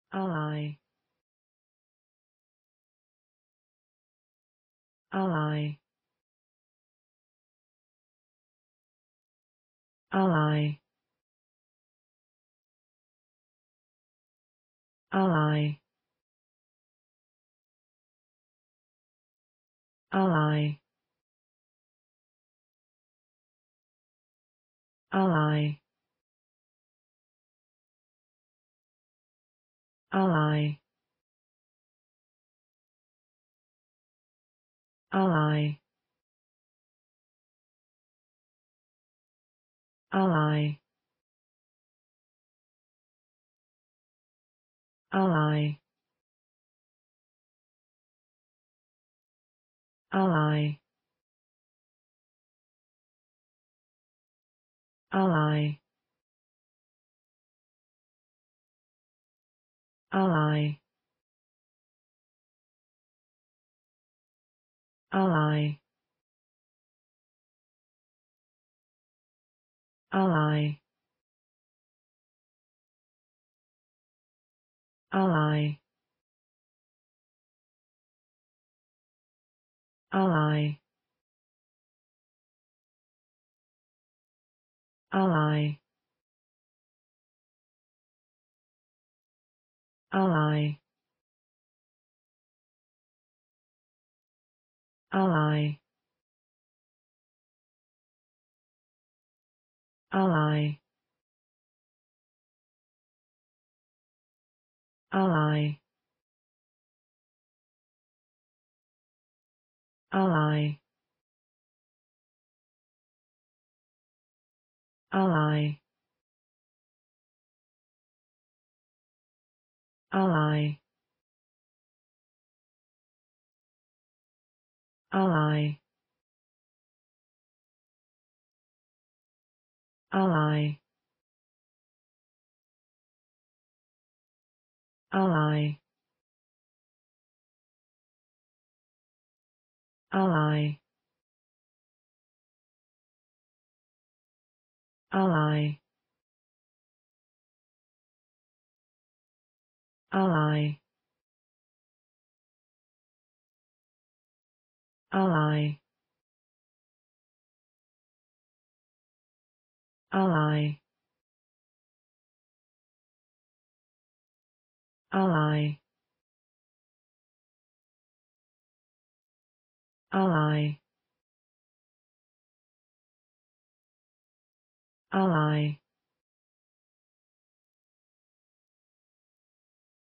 how to pronounce the english word Alai.amazing resource.learn from me.
Follow me!Learn from me how to pronounce the english word Alai.This video shows you how to pronounce the English word  Alai .  the word means   阿莱山脉 or 阿萊山脈 or a lai shan mai.